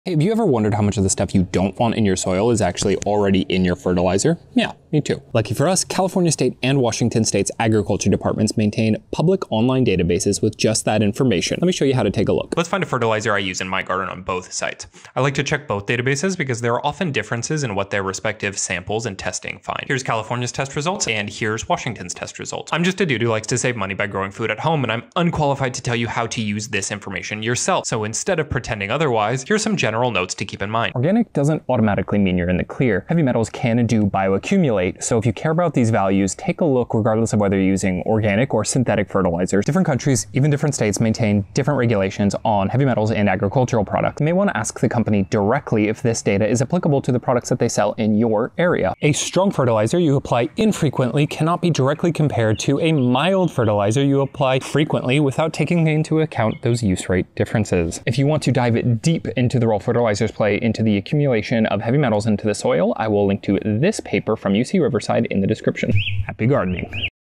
How to Compare Heavy Metals in Garden Fertilizers - *Garden Quick Tips*
As gardeners, we know how to immediately check a fertilizer for key plant nutrients like nitrogen, phosphorus, potassium, calcium, etc. But what about things like lead, cadmium, arsenic, and more?

We're in luck, because both California and Washington state's agriculture departments maintain incredibly useful - and totally free/public/online - databases of the test results they run on all commercial fertilizers sold in their states.

UC Riverside Paper on Fertilizer Impact on Accumulation of Heavy Metals in Soil